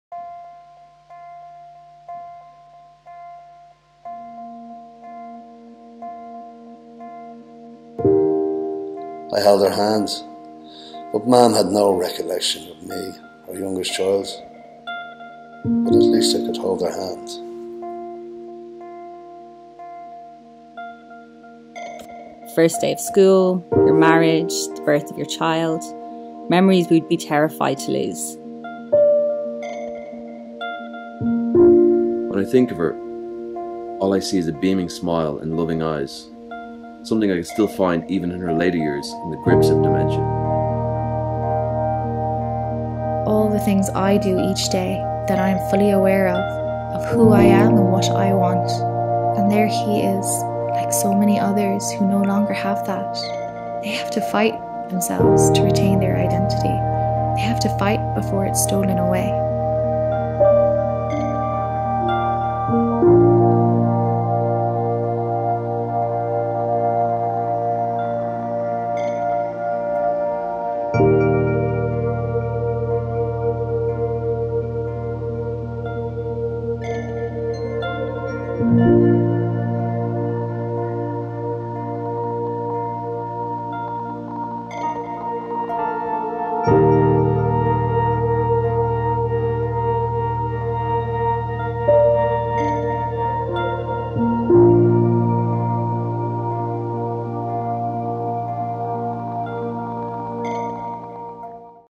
In collaboration with irish filmmaker Tony Whelan of Canola Pictures, the ARTzheimer's story is being told through the eyes of some of our wonderful and talented artists.